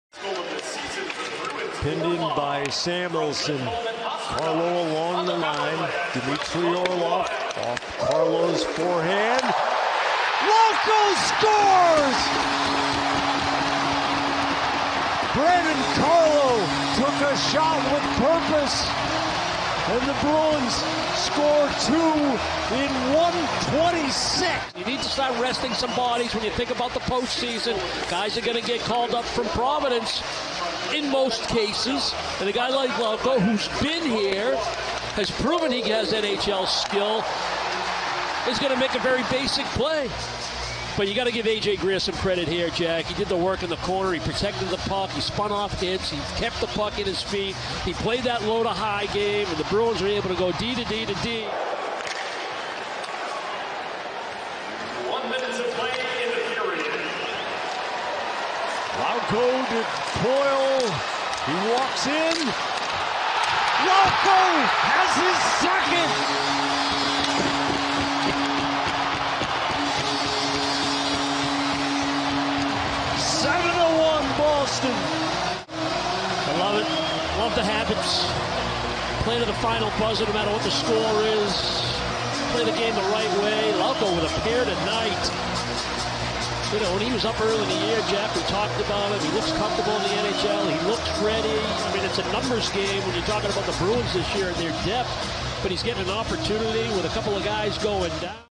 Jakub Lauko scores two goals vs Sabres (2 mar 2023)
Laukovy 2 góly v utkani s Buffalem.